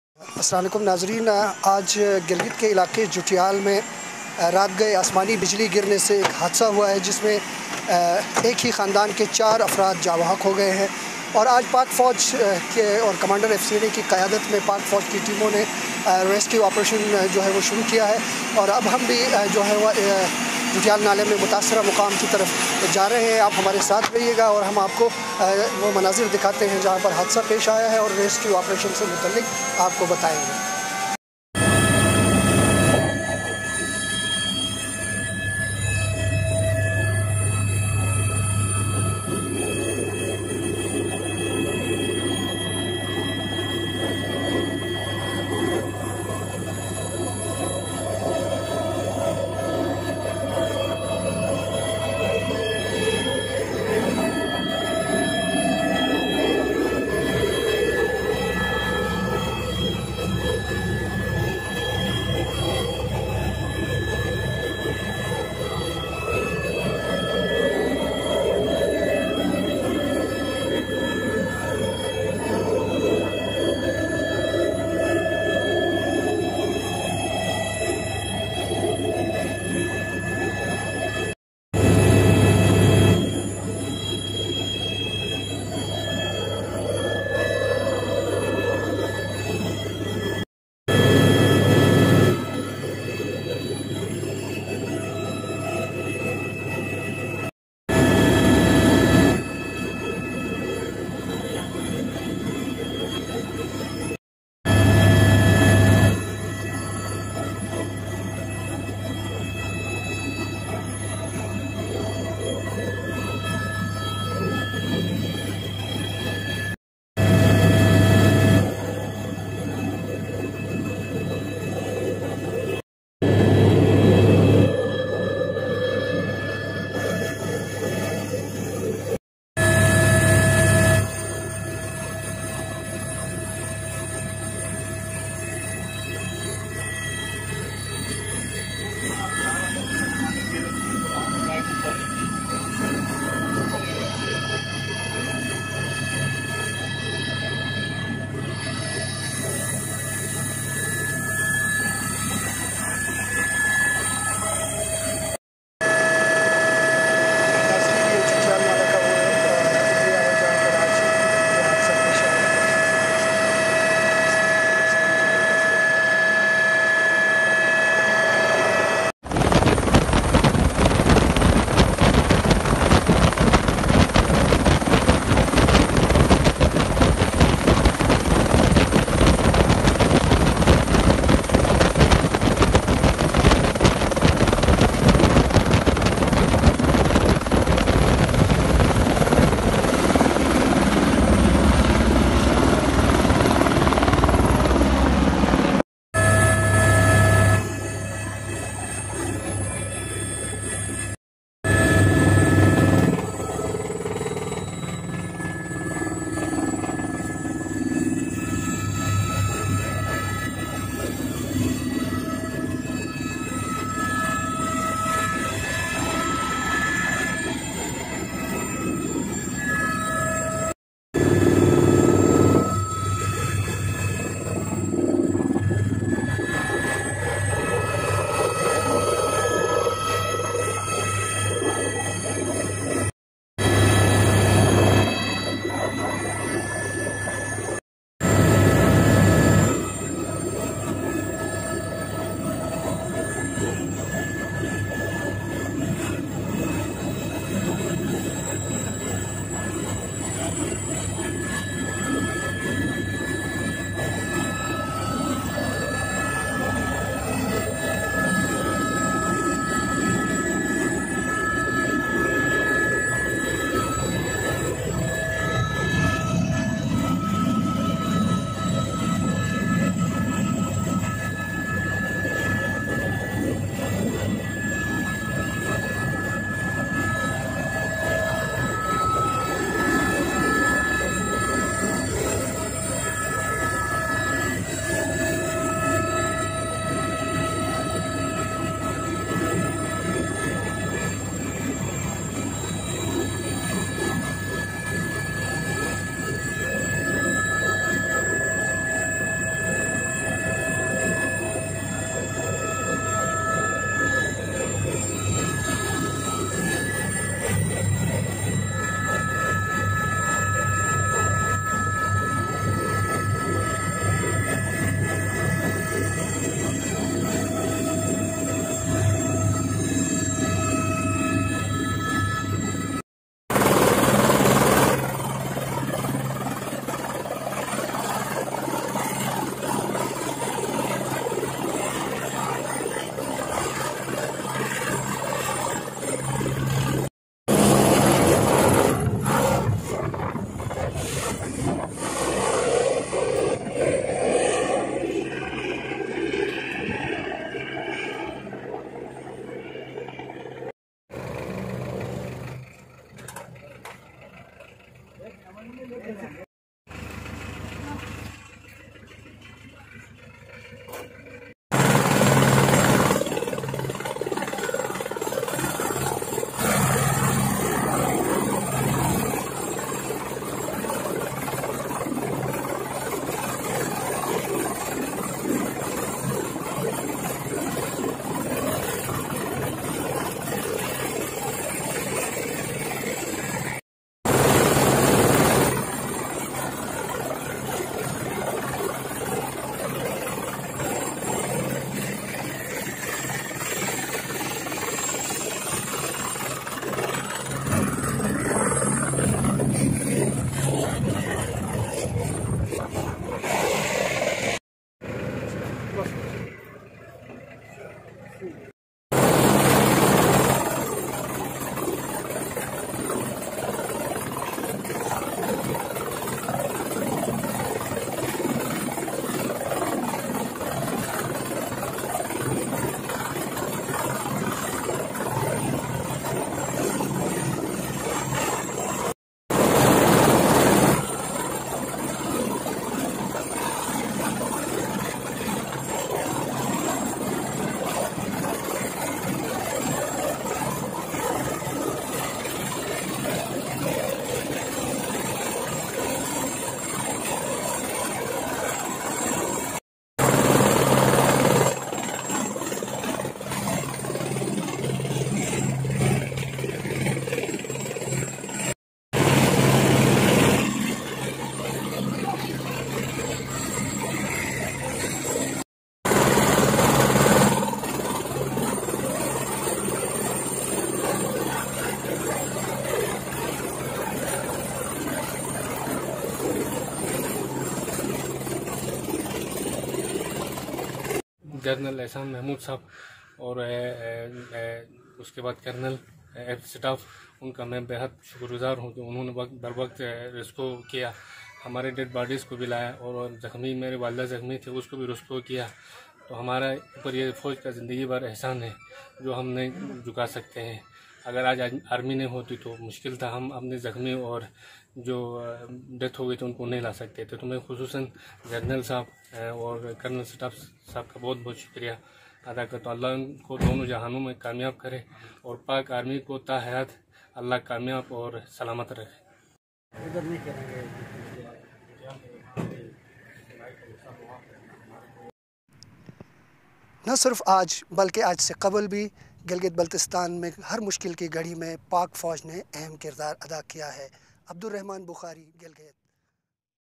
جوٹیال نالہ پاک فوجریسکیو آپریشن
Choppers of Pakistan Army have evacuated the dead bodies of four members of a family killed by lightning in the mountains of Jutial, # Gilgit.